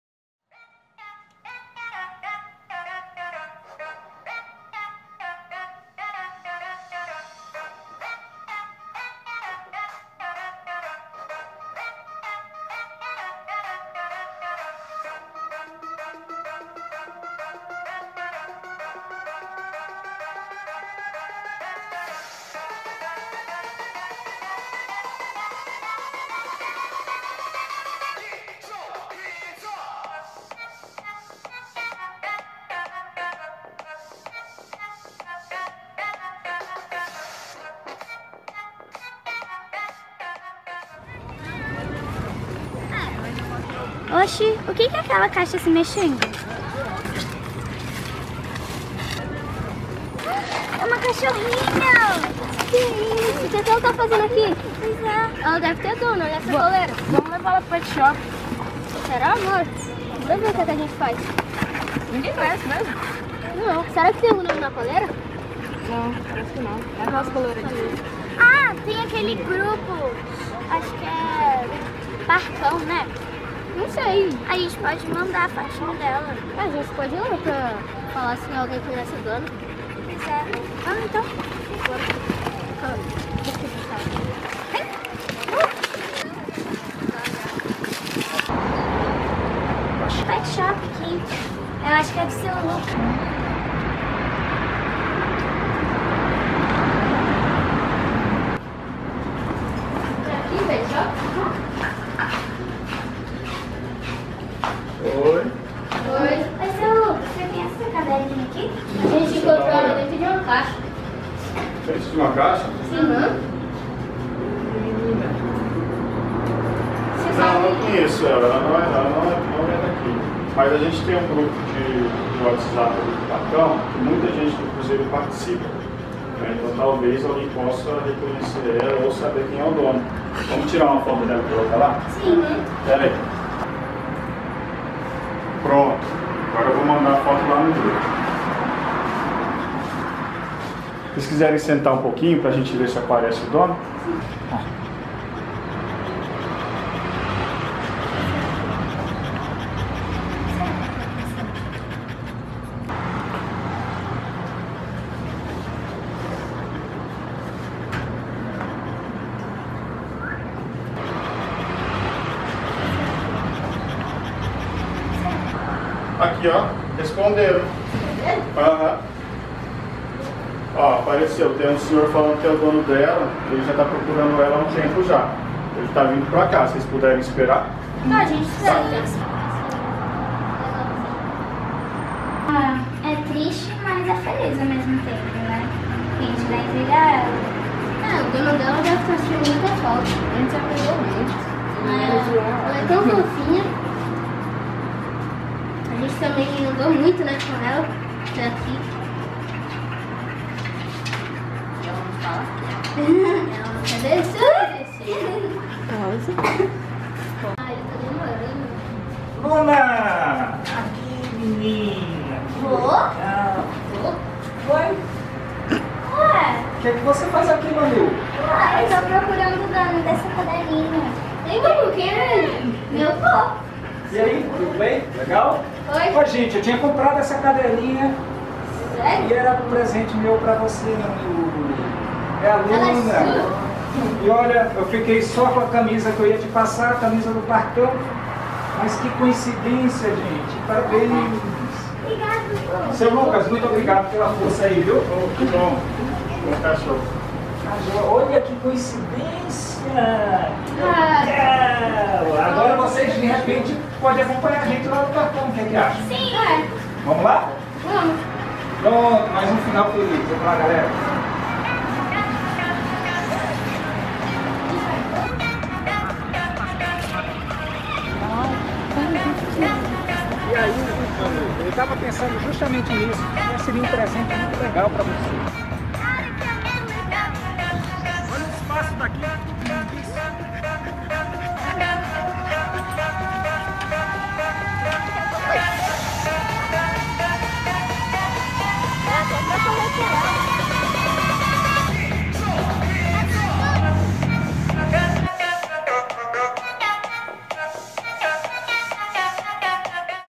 7C A CADELINHA PERDIDA
No meio a um passeio, uma surpresa: encontraram uma cadelinha! que fazer para devolvê-la ao dono?

Vídeo vencedor do Troféu CRUZEIRITO 1o Lugar entre os sétimos anos no 5o Festival CURTA um CURTA do CEF 1 do Cruzeiro, em 2019.